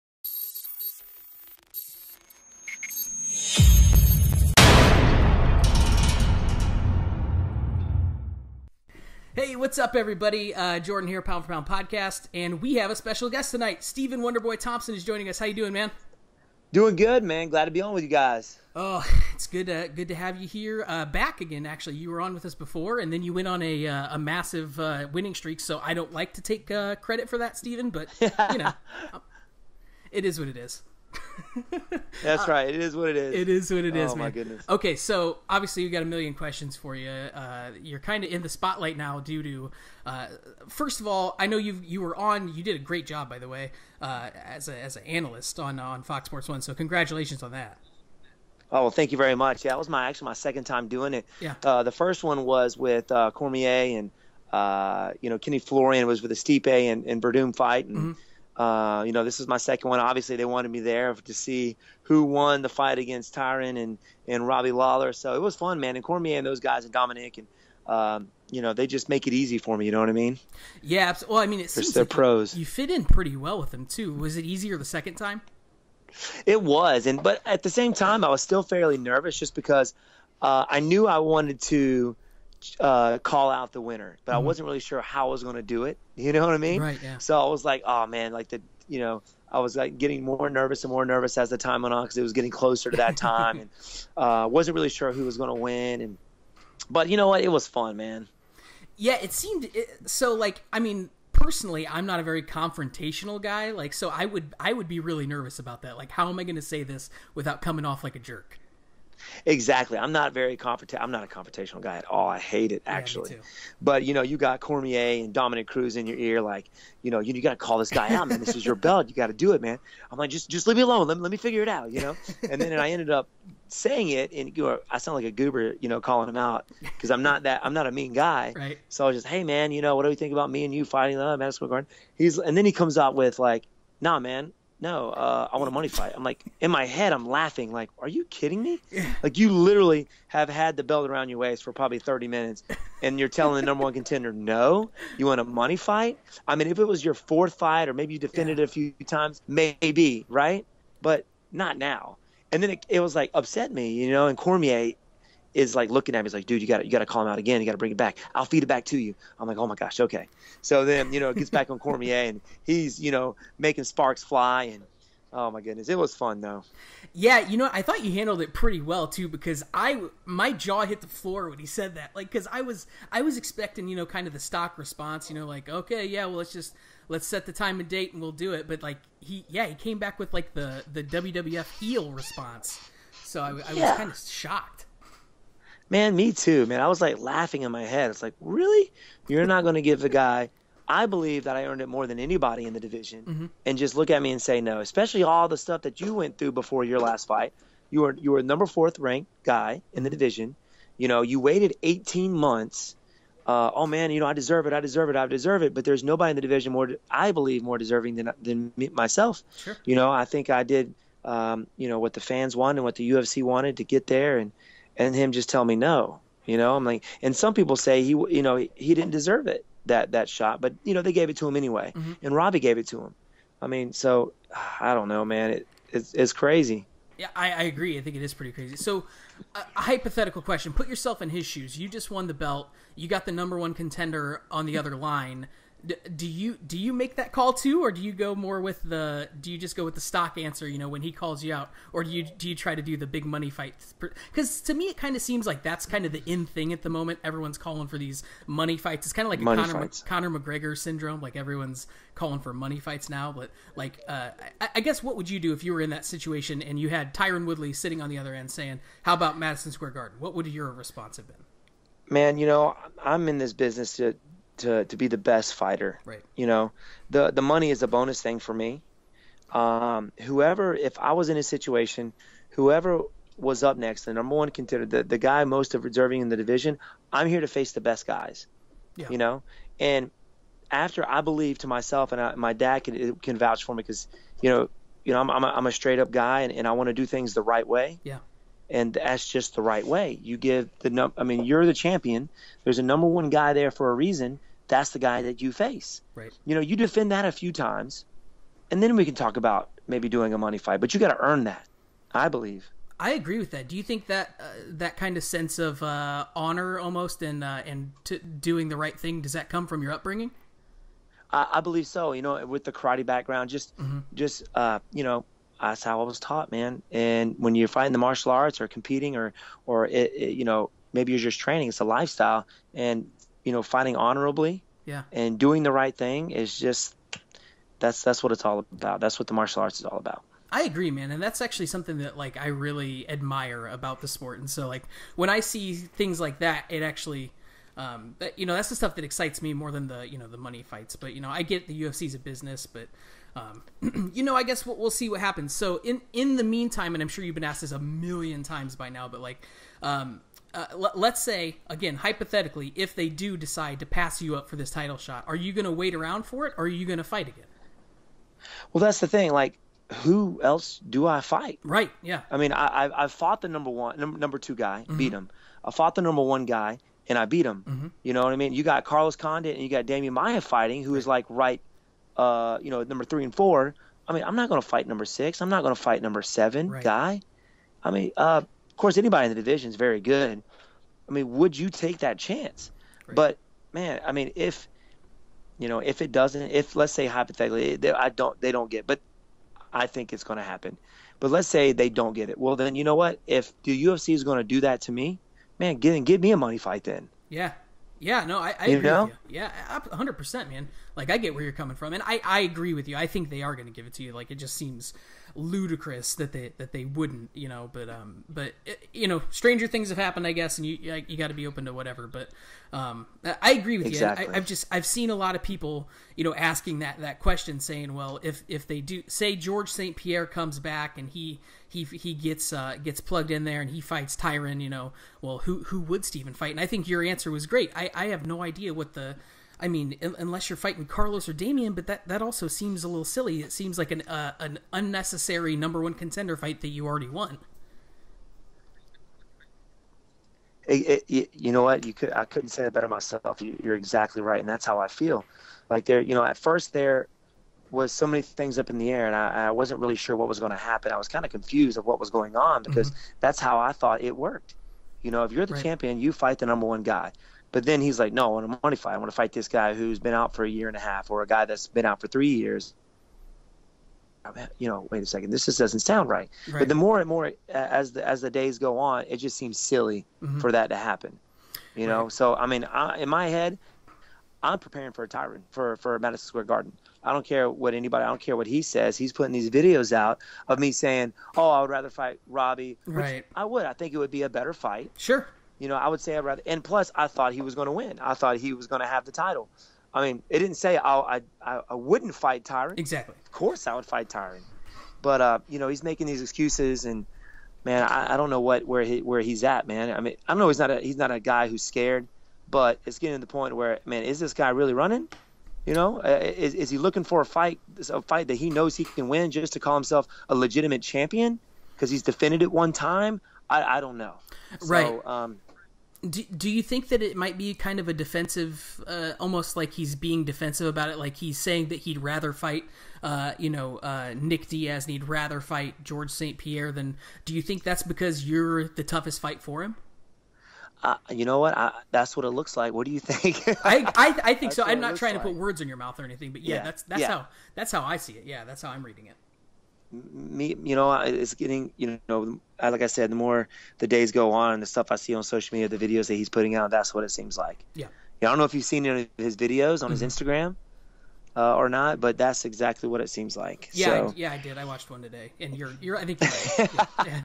Wonderboy: I Am The Contender, I Earned A Shot! | Tyron Woodley | MMA Latest
Stephen 'Wonderboy' Thompson spoke to Jordan on our P4P podcast this week and he outlined the reasons he deserves a shot at Welterweight Champion Tyron Woodley.

Check out the P4P podcast (and all our other podcasts) here;